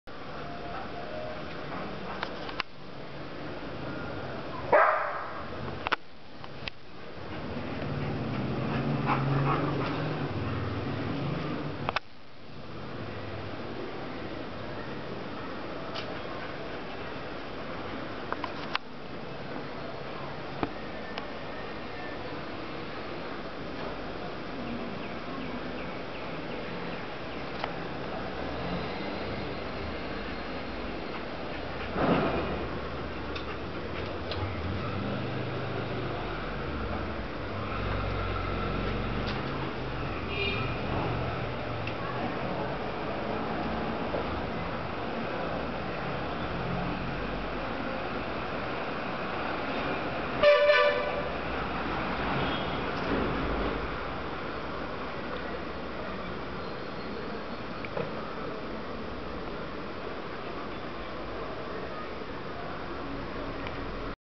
Tasha's (GSD) favourite self play
Tasha is so disciplined that she hates dogs who are off leash. She will bark incessantly, run up and down the compound to chase them away.